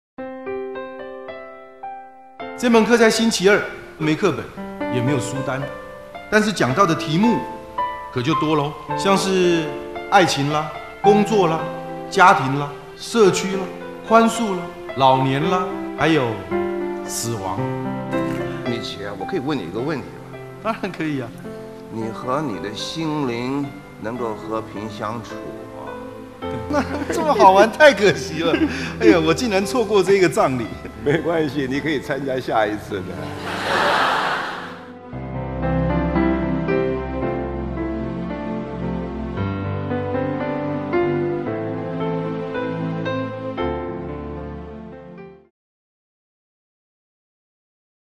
Tuesdays With Morrie (20 – 22 Jul 2018)
by Godot Theatre Company (Taiwan)

“The most important thing in life is to learn how to give out love, and to let it come in.”

One of the world’s top selling memoirs of all time, Tuesdays with Morrie is a moving rumination about life and death, based on author Mitch Albom’s interviews with his college Professor Morrie Schwartz. Schwartz was afflicted with the terminal ALS disease (Lou Gehrig’s disease) and the story chronicles his thoughts about living a meaningful life over a series of meetings—always on Tuesdays—that Schwartz had with Albom before his passing. This heartwarming theatrical adaptation of the book features Golden Horse Award-winner Jin Shi Jie as Morrie and popular host Pu Xueliang as Mitch.

Performed since 2011 with over 200 shows and highly recommended by 200,000 audience, this gentle, charming and thought-provoking play is full of vitality and has touched the lives of many.